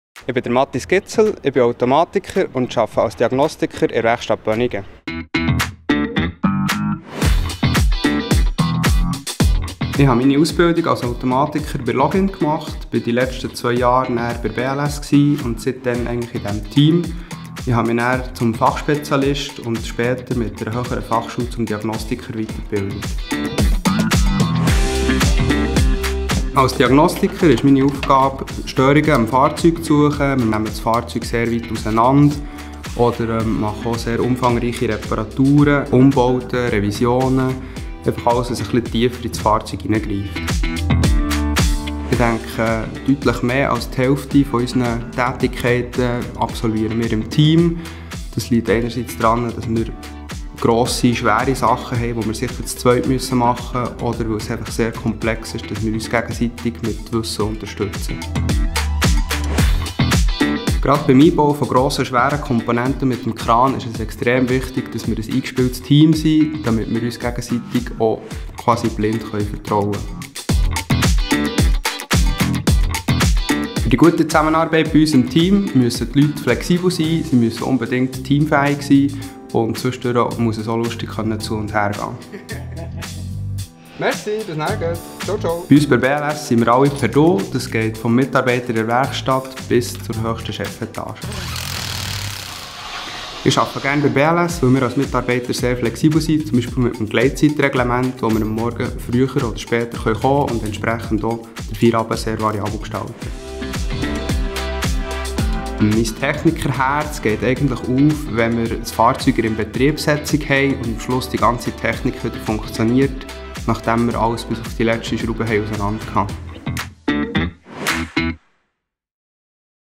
Arbeiten in der Werkstatt: Mathis
Mathis führt die Lok Re465 durch ein Modernisierungsprogramm. Dabei greift er tief ins Fahrzeug ein und stellt sich vielseitigen technischen Herausforderungen. In jeder Situation kann er sich auf seine Kollegen verlassen.